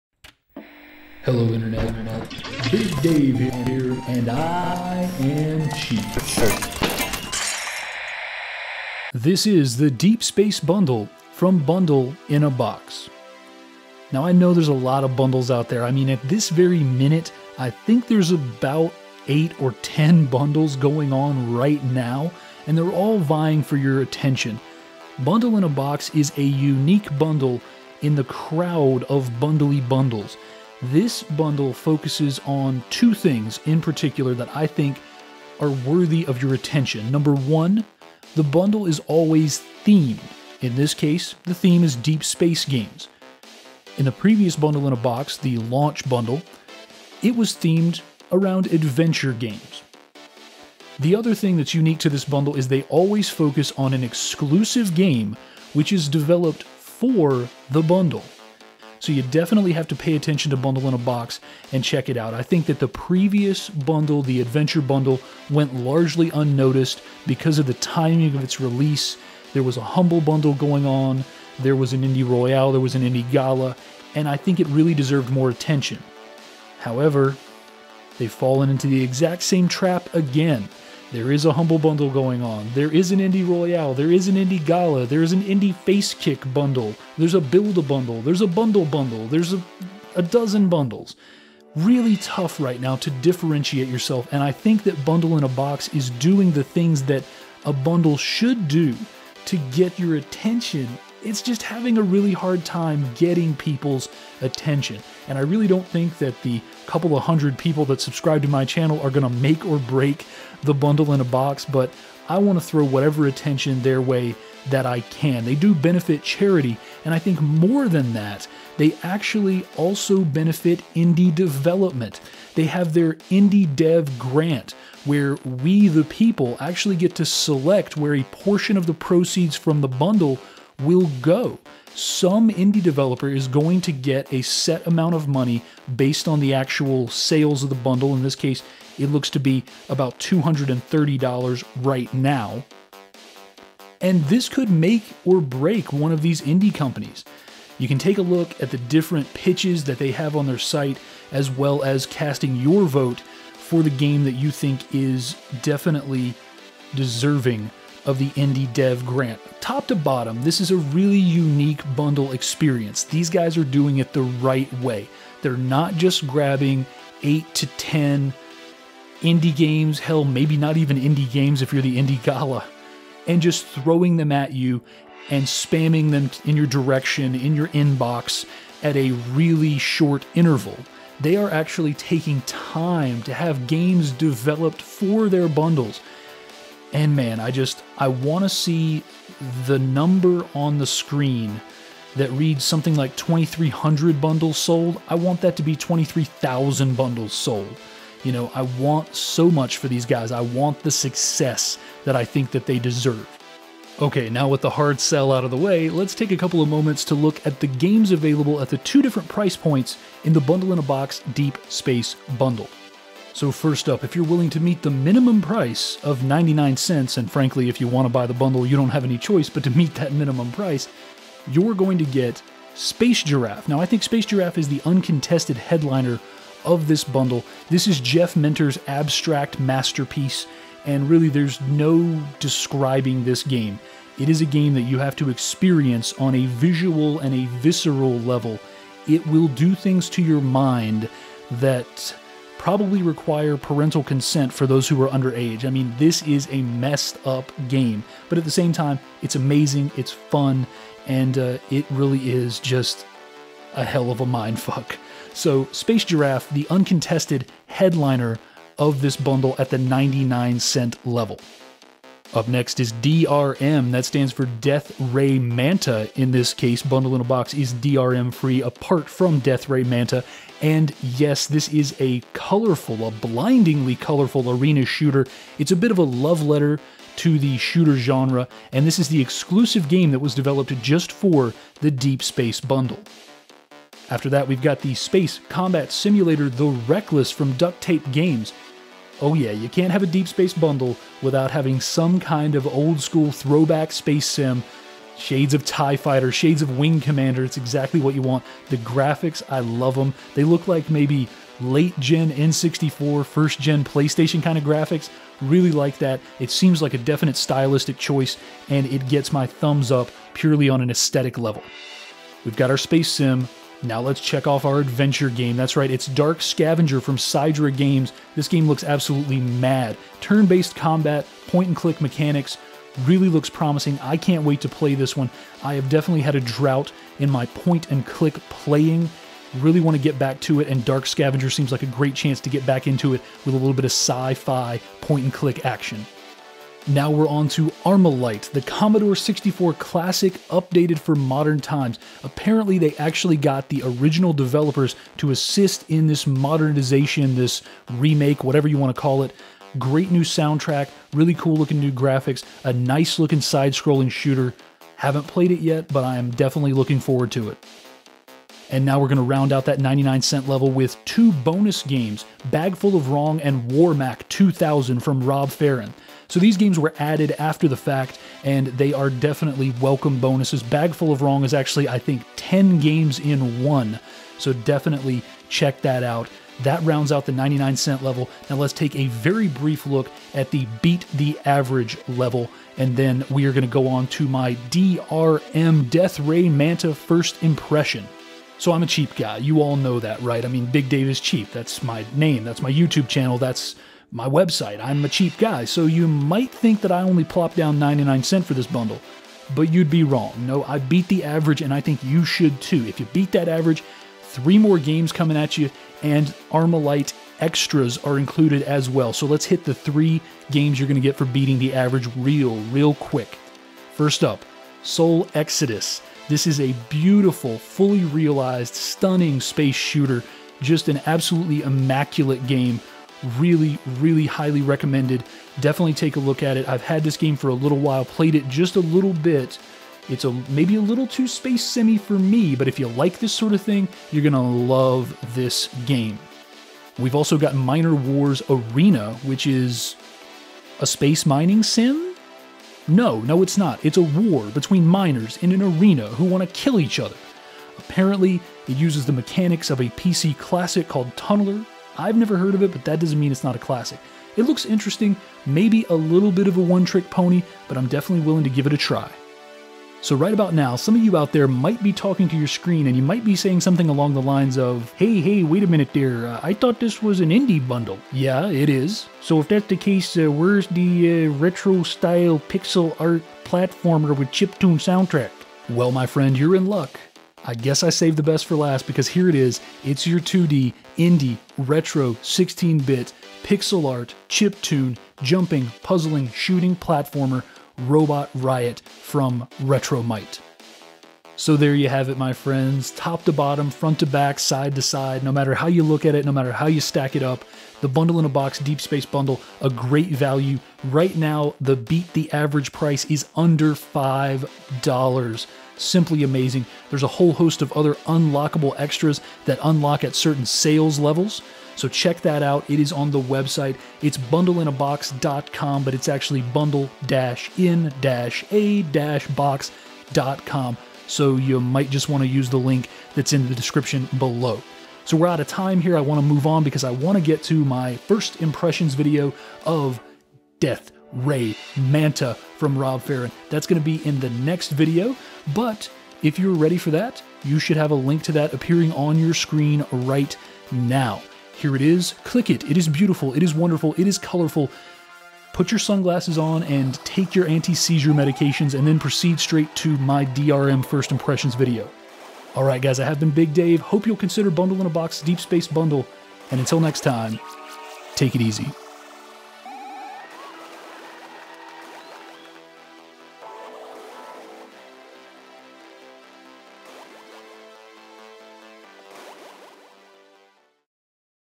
Bundle in a Box - Deep Space
Big Dave takes a look at the latest offering from Bundle in a Box it's the Deep Space bundle!